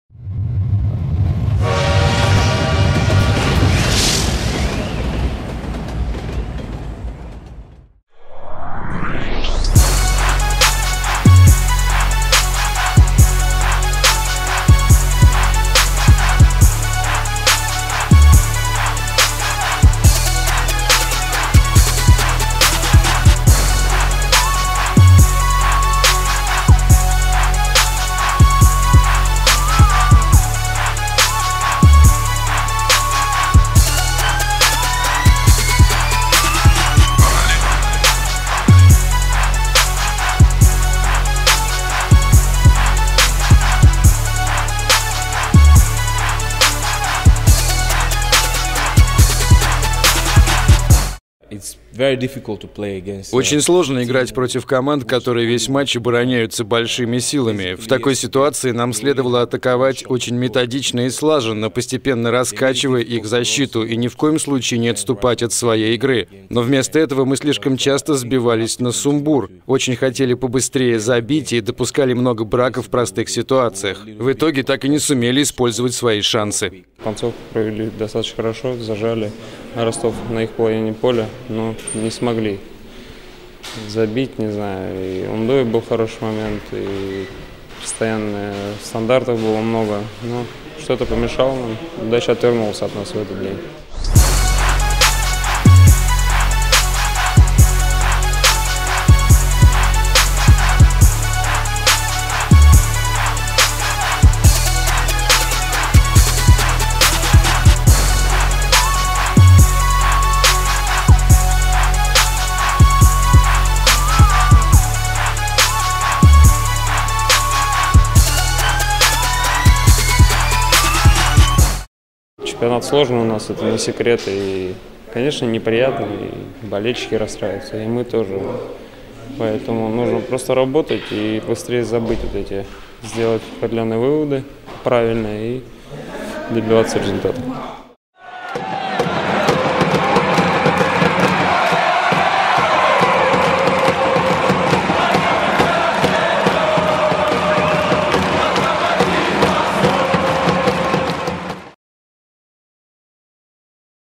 «Нужно сделать верные выводы и добиваться результата»
Полузащитники «Локомотива» Дмитрий Тарасов и Виктор Обинна, вернувшись с выезда в Ростов-на-Дону, после восстановительной тренировки ответили на вопросы клубного телевидения. Локо-ТВ